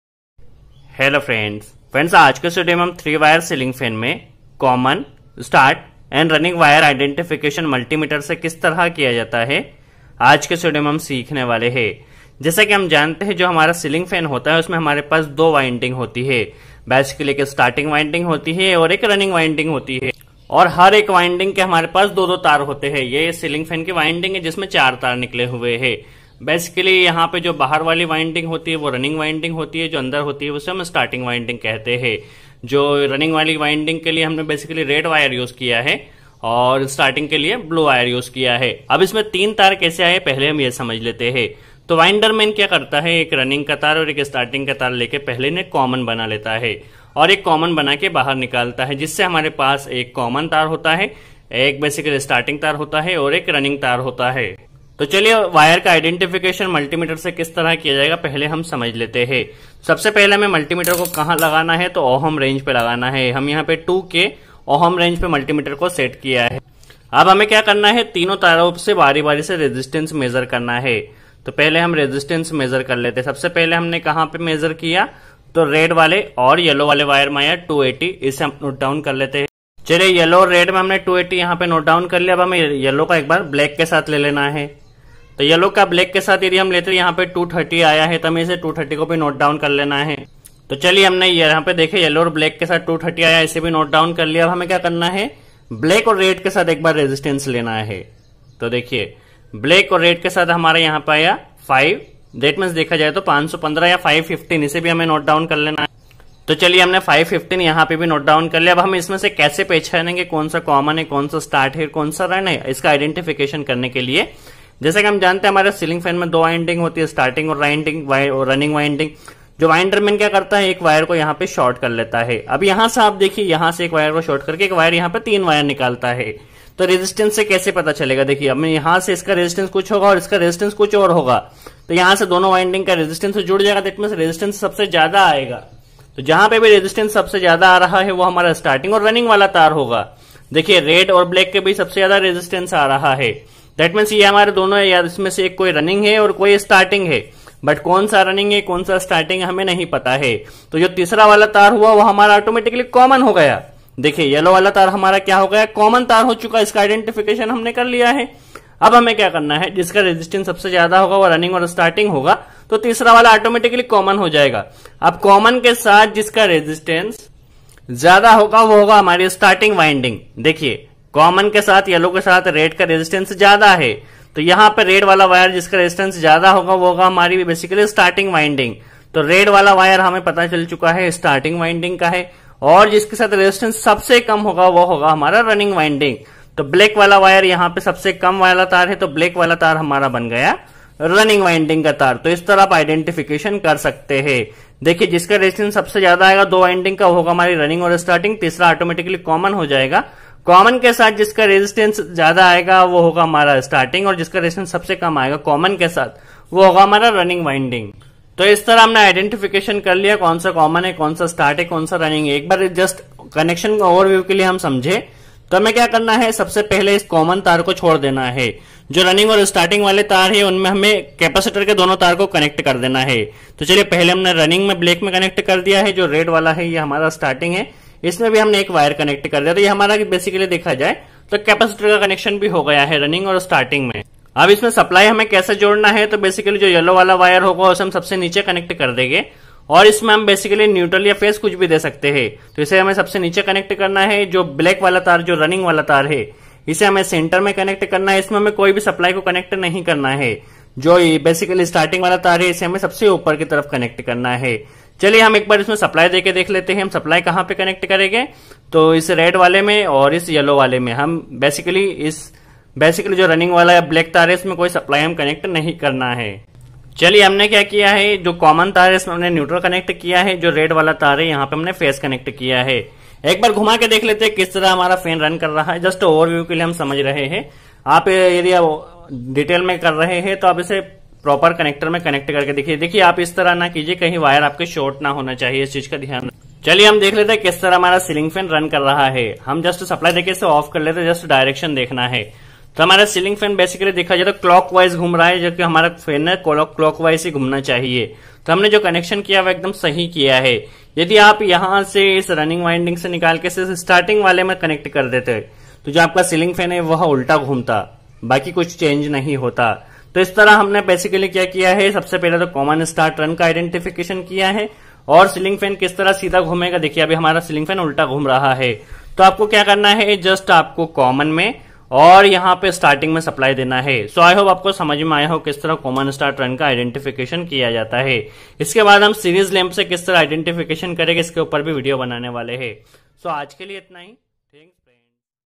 CEILING FAN मे COMMON, START, RUN WIRE MULTIMETER से कैसे पता करे! HOW TO IDENTIFY CEILING FAN WIRE
3 wire ceiling fan common start run find

Is video me hmne ceiling fan me common start run wire ko multimeter se kese find kiya jata h

Hmne sikha h basically fan me २ winding hoti h starting winding & running winding

To is video me hmne common start run find krne ka kafi aasan tarike se sikhya h